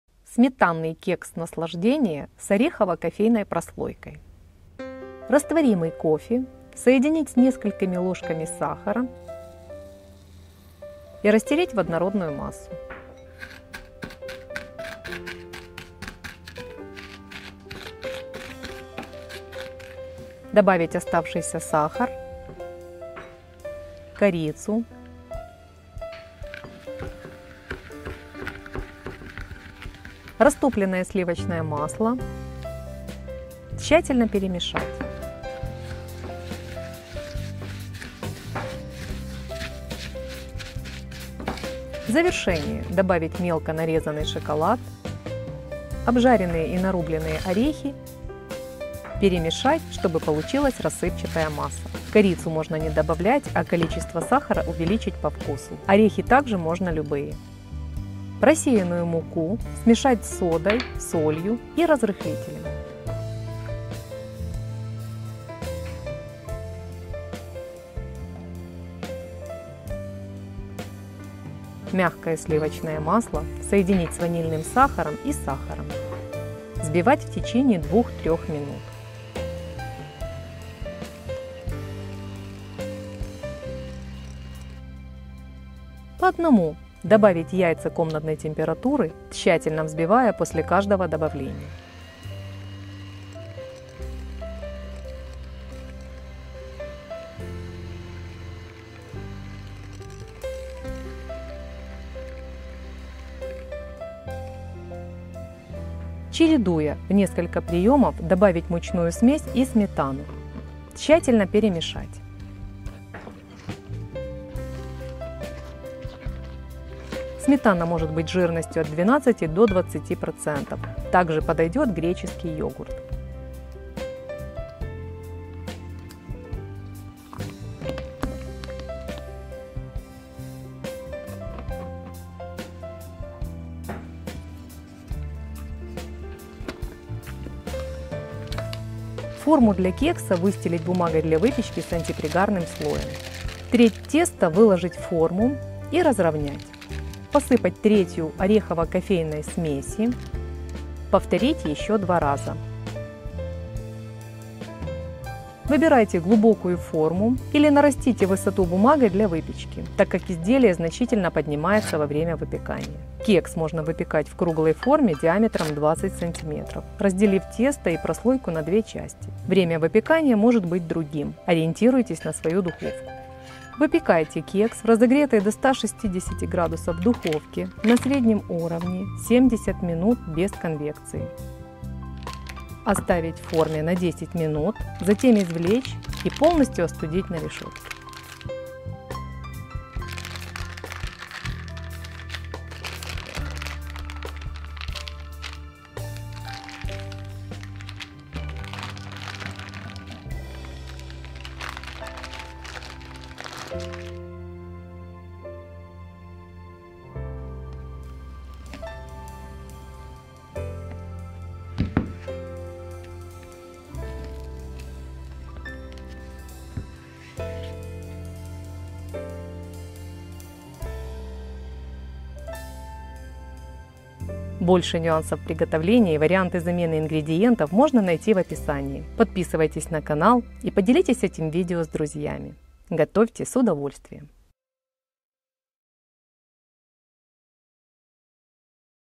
Сметанный кекс "Наслаждение" с орехово-кофейной прослойкой
Очень вкусный и простой в приготовлении кекс с богатым вкусом.

Орехи подойдут любые, главное условие - обжарить перед нарезкой, чтобы лучше раскрылся вкус.
Кофе выбирайте по вкусу, удобнее всего использовать маленькие порционные пакеты/стики.

Сода обязательно добавляется в тесто, когда используем кисломолочный продукт, фруктовый (овощной) сок/пюре, кусочки фруктов, мед, большое количество натурального какао, чтобы нейтрализовать кислоту, содержащуюся в этих продуктах. Если этого не сделать, то консистенция готового изделия будет клеклой.

Подъемной силы от этой реакции не хватит для роста теста, поэтому добавляется разрыхлитель, так как он обеспечивает подъем выпечки, реагируя в тесте без остатка.

Сметана может быть жирностью от 12 до 20%, так же подойдёт греческий йогурт.
Корицу можно не добавлять, а количество сахара увеличить по вкусу.

Выбирайте глубокую форму, или нарастите высоту бумагой для выпечки, так как изделия значительно поднимается во время выпекания.
Кекс можно выпекать в круглой форме диаметром 20, разделив тесто и прослойку на 2 части. Время выпекания может быть другим, ориентируйтесь на свою духовку.

Ингредиенты:
Яйца - 3 штуки среднего размера (150-160 г без скорлупы)
Сметана 15-20 % - 240 г

Орехово-кофейный слой
Сахар - 80 г
Кофе растворимый  - 2-4 г
Шоколадная стружка - 50 г
Сливочное масло - 30 г
Корица - 4 г
Орехи грецкие - 100 г

Приготовление:

1. Форму для кекса выстрелить бумагой для выпечки с антипригарным слоем.
2. Растворимый кофе соединить с несколькими ложками сахара и растереть в однородную массу.
Добавить оставшийся сахар, корицу, растопленное сливочное масло, тщательно перемешать. В завершение добавить мелко нарезанный шоколад, обжаренные и нарубленные орехи, перемешать, чтобы получилась рассыпчатая масса.
3. Просеянную муку смешать с содой, солью и разрыхлителем
4. Мягкое сливочное масло соединить с ванильным сахаром и сахаром  взбивать в течение 2-3 минут
4. По одному добавить яйца комнатной температуры, тщательно взбивая после каждого добавления.
5. Чередуя, в несколько приемов, добавить мучную смесь и сметану, тщательно перемешать. Сметана может быть жирностью от 12 до 20%, так же подойдёт греческий йогурт.
6. Треть теста выложить в форму и разровнять, посыпать 1/3 орехово-кофейной смеси, повторить еще два раза, верхним слоем должна оказаться ореховая смесь.
7.  Выпекать кекс в разогретой до 160°С духовке на среднем уровне 70 минут без конвекции.
8. Оставить форме на 10 минут, затем извлечь и полностью остудить на решетке.

*Обратите внимание!
Время выпекания может отличаться от заявленного в видео, ориентируйтесь на свою духовку, а так же учитывайте материал, из которого изготовлена форма.